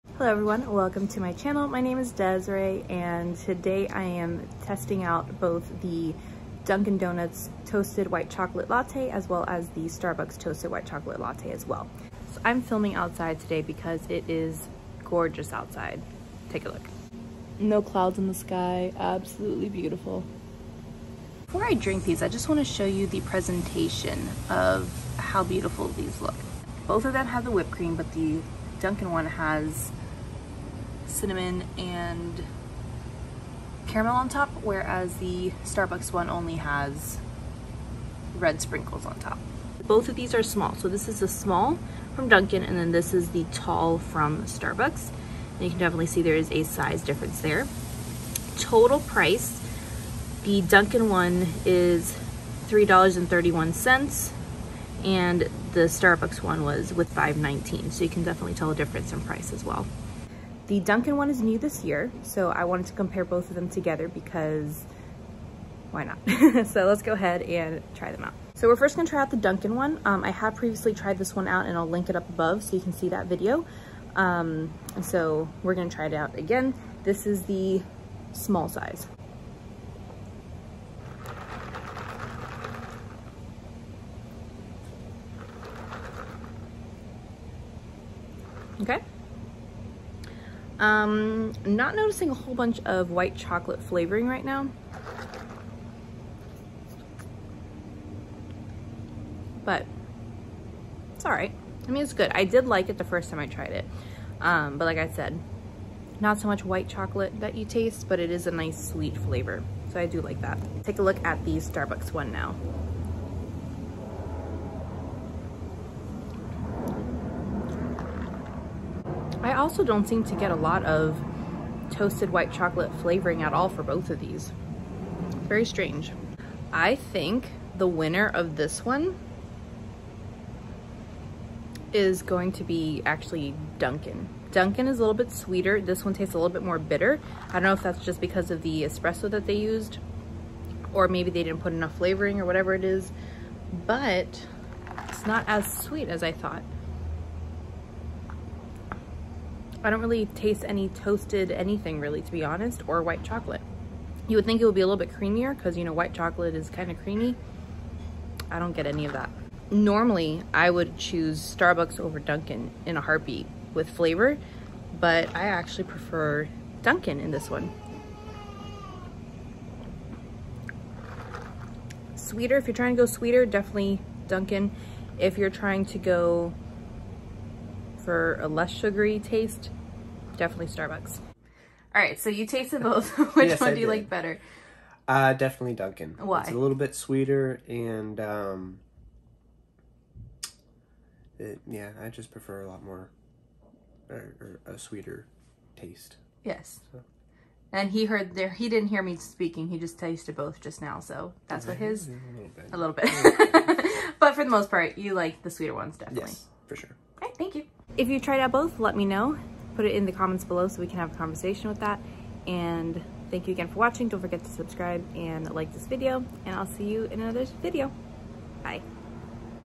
Dunkin’ Donuts vs Starbucks Toasted White Chocolate Iced Latte | Holiday 2021 Taste Test
Hi Coffee Friends! Today I taste tested the Dunkin Donuts Toasted White Chocolate iced latte and compared it to the Starbucks Toasted White Chocolate iced latte. Let me know which one you prefer in the comments below!